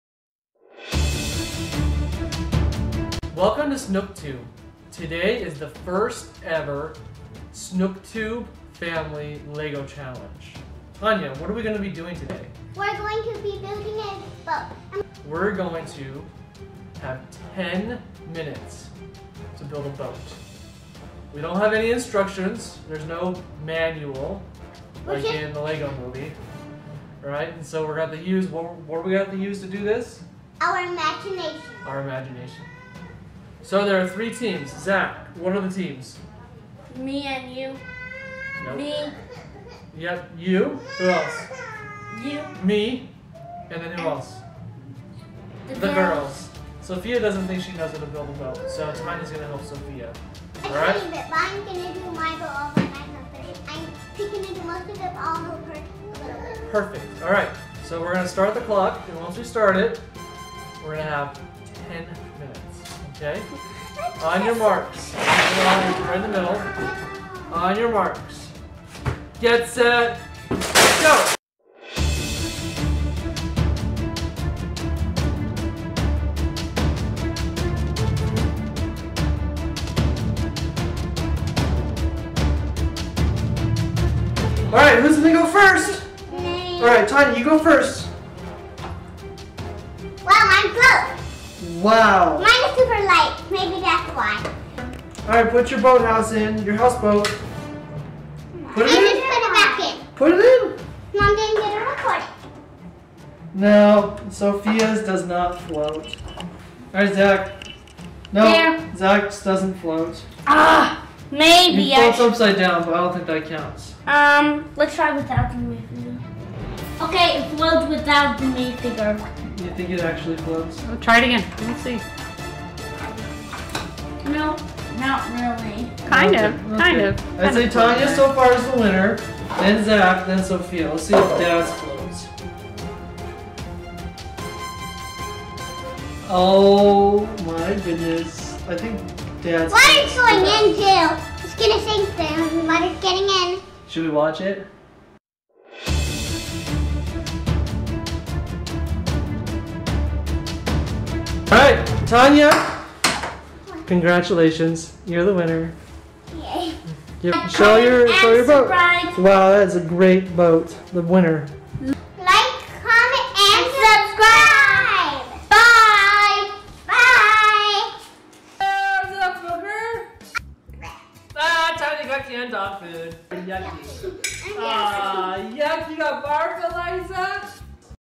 Family Lego Boat Challenge!
Join the Snook Family in a Lego boat Challenge! No manuals. No guides. Just creative power! And they will put their boats to the test in water.